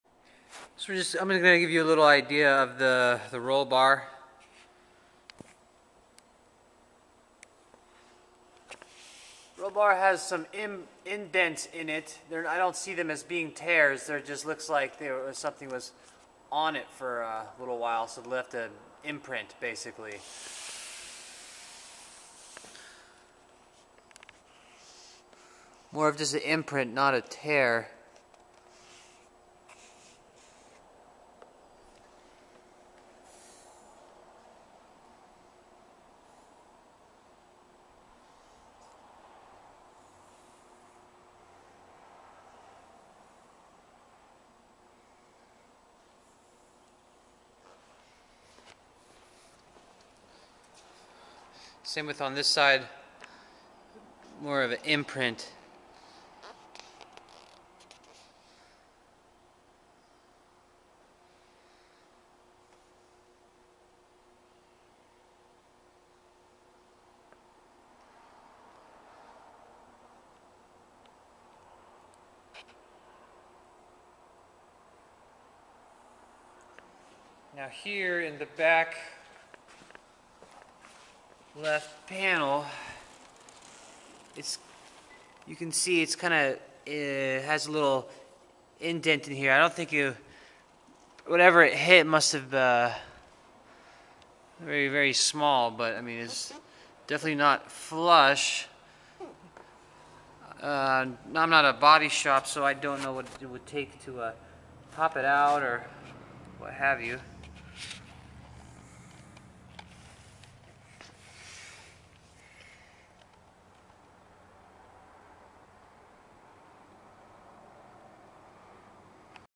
1998 Mercedes SL600 Imperfections
Roll Bar has few minor indents. Rear Left quarter panel has small dent.